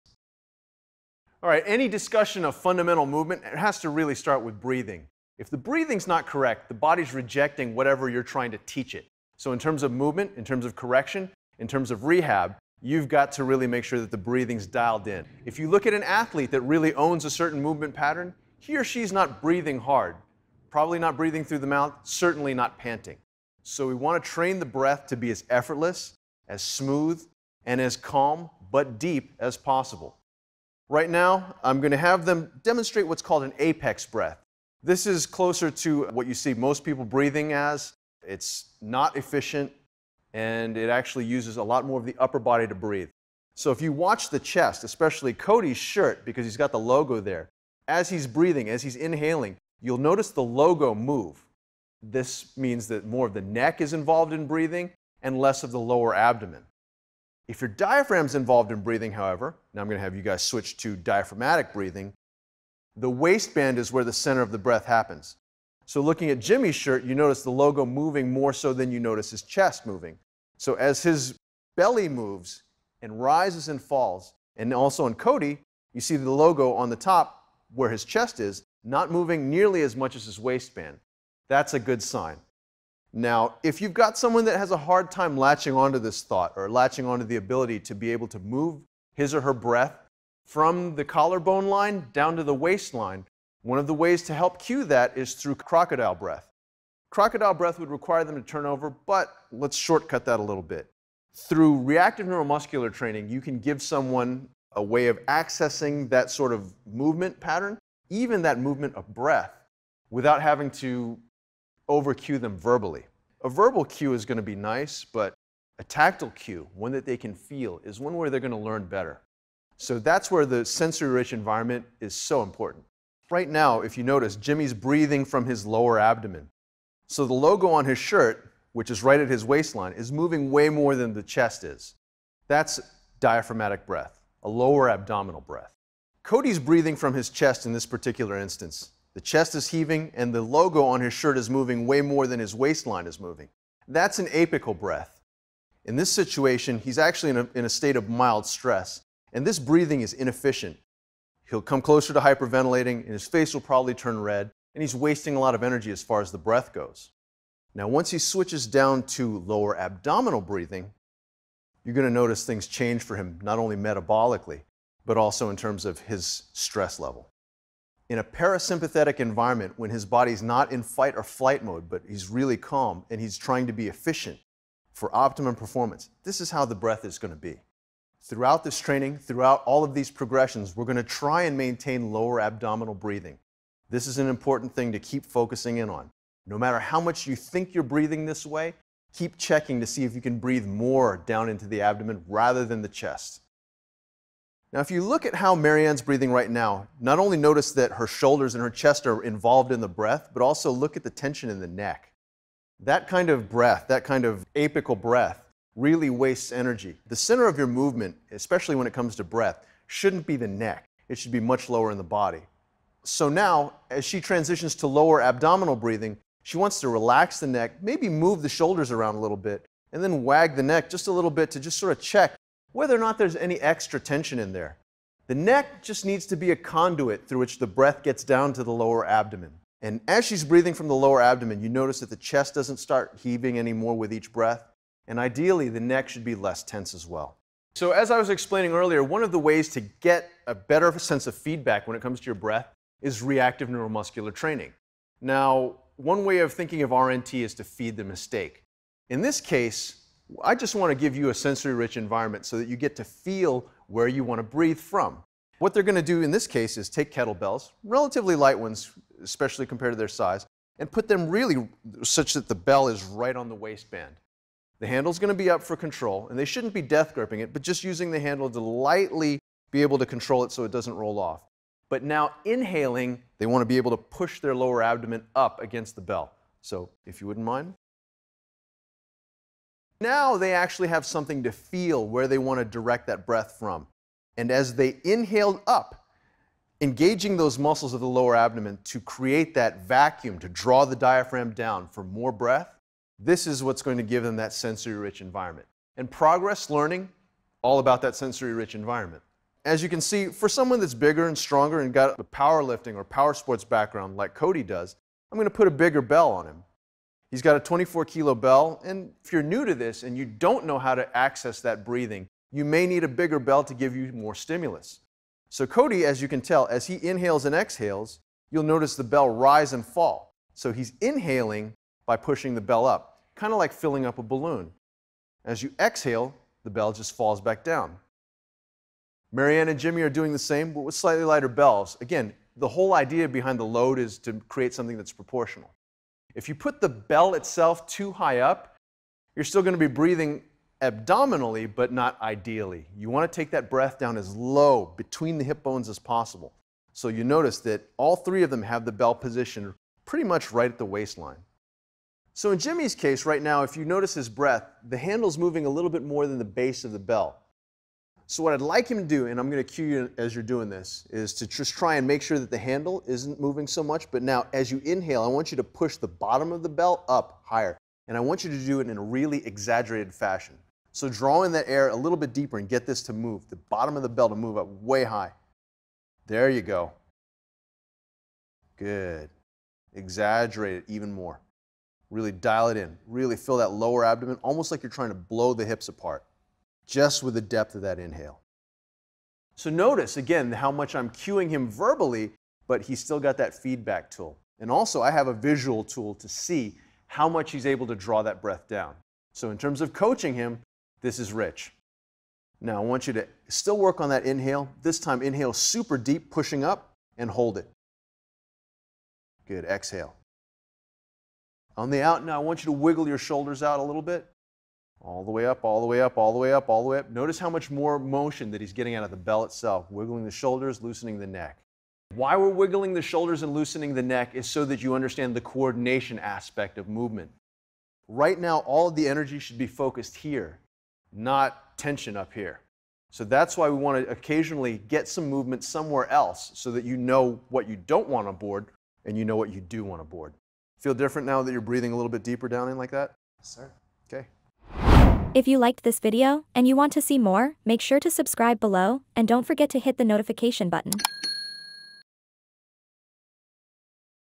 Optimise Your Performance: Dr. Mark Cheng's Insights into Athletic Breathing
In this excerpt from Dr. Mark Cheng's lecture series "Prehab Rehab 101," Dr. Cheng delves deep into the essence of "good" breathing. Drawing from his vast experience in rehabilitation, sports medicine, and martial arts, he presents an in-depth guide for trainers to teach their athletes the four fundamental qualities of optimal breathing. Whether you're a coach, athlete, or fitness enthusiast, this video offers invaluable insights into harnessing the power of breath for peak performance and holistic well-being. Don't miss out on these evidence-based techniques and transformative breathing strategies that can set the foundation for optimal athletic performance!